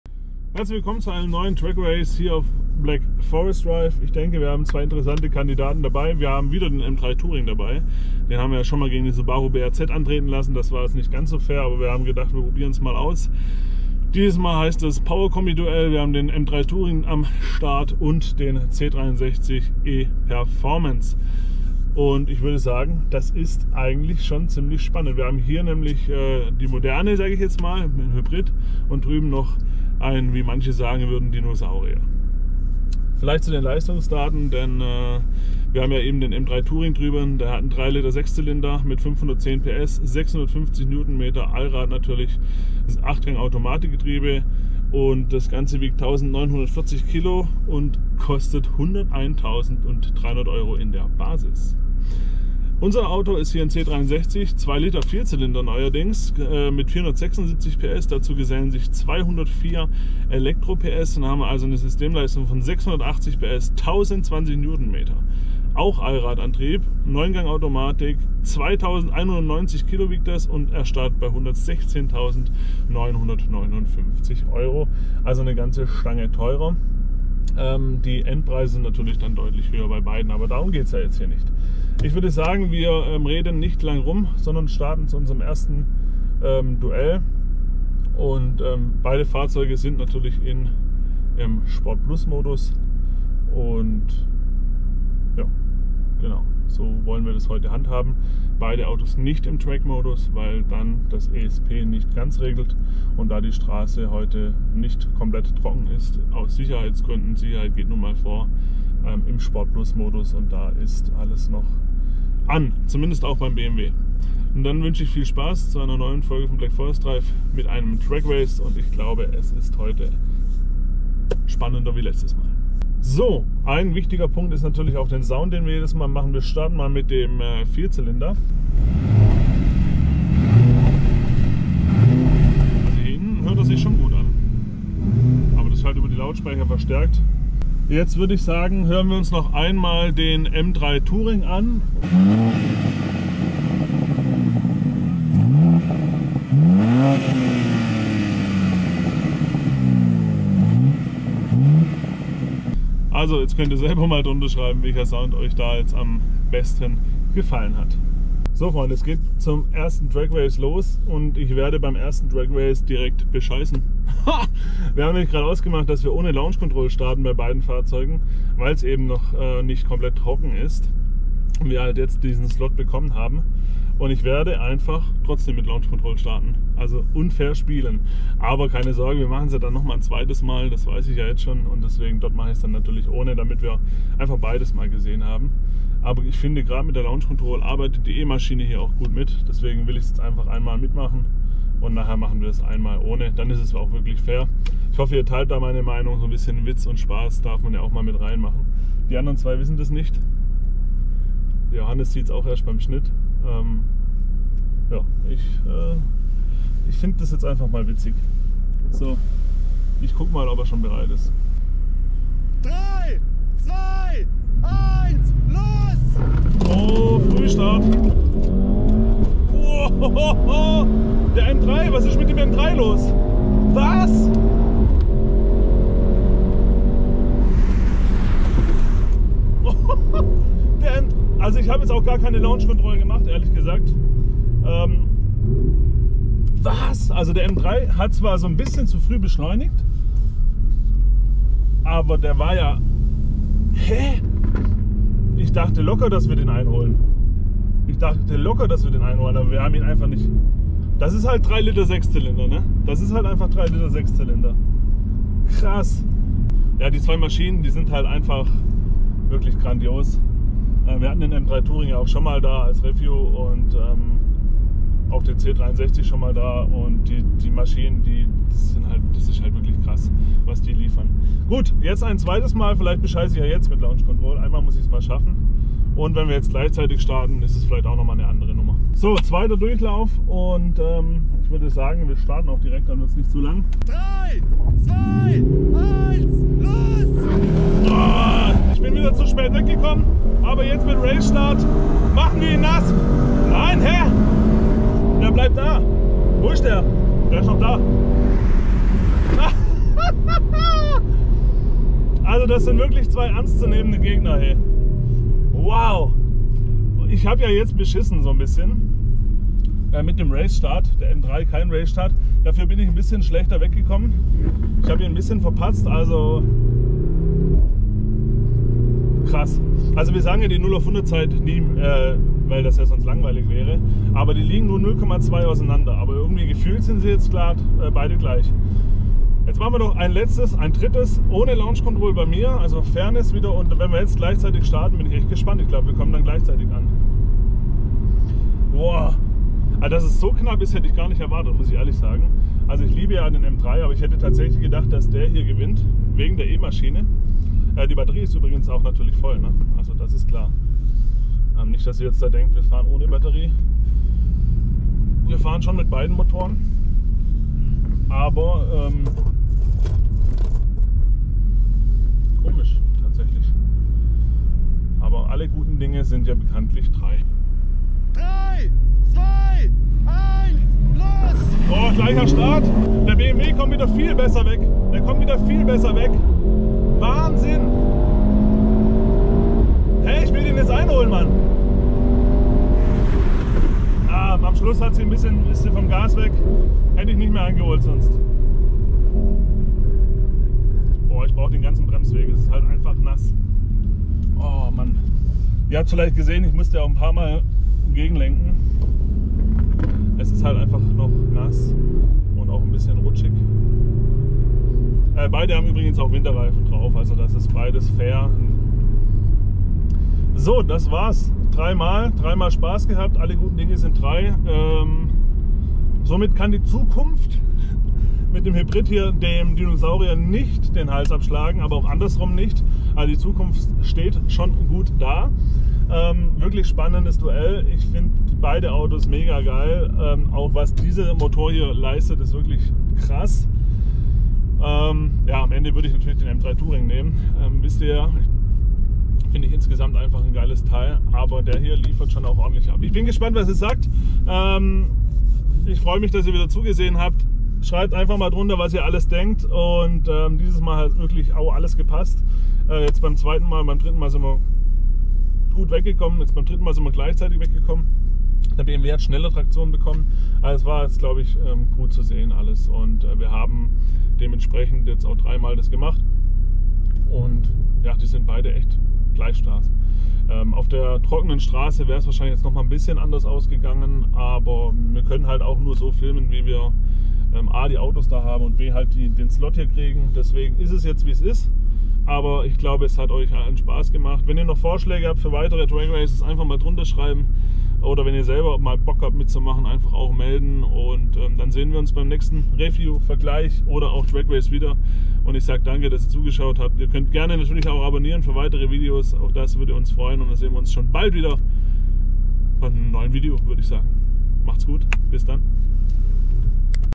BMW M3 Touring 🆚 Mercedes AMG C 63 S E-Performance - Drag Race
Viel Spaß bei diesem DRAG RACE mit dem BMW M3 Competition Touring gegen den Mercedes-AMG C 63 S E Performance. Wir hoffen es gefällt euch und Ihr habt Lust auf mehr :)